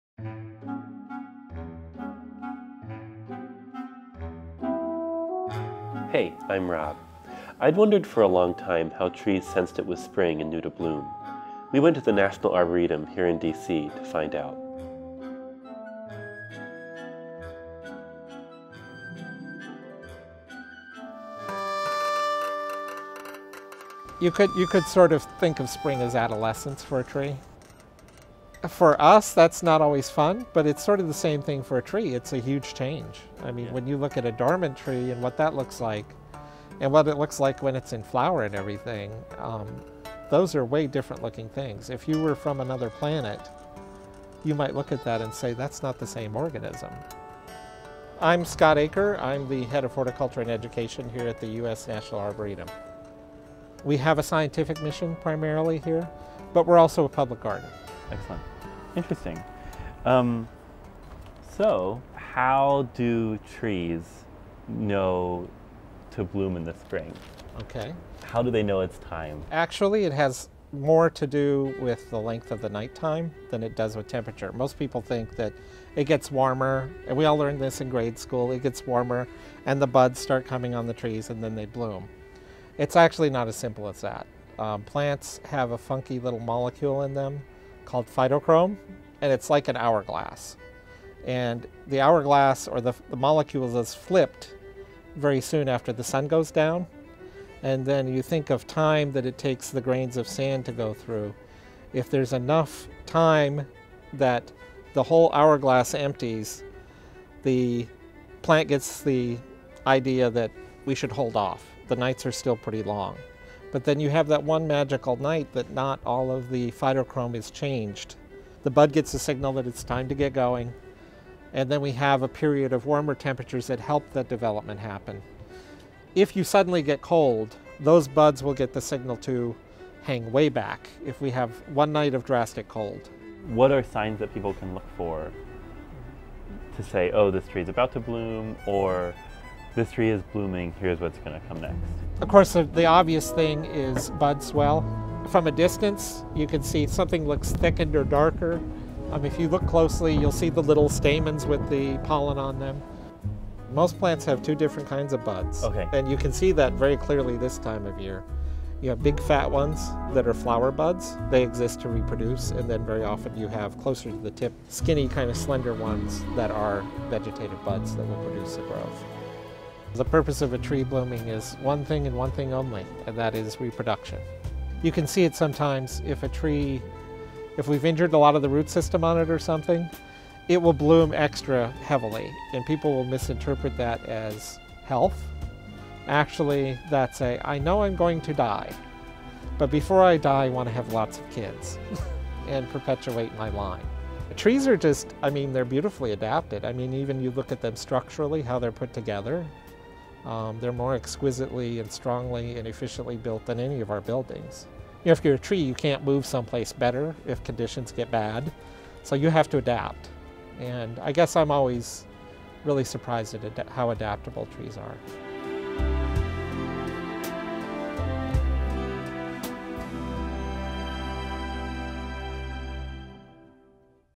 How Do Trees Know When It's Spring?
It’s a warm, sunny day in April, and all the trees are in bloom. But how did they know it was warm and sunny? And on a warm, sunny day in February, what keeps them from blooming? Scott Aker, a horticulturalist at the U.S. National Arboretum, tells Robinson Meyer how the process works and what to look for.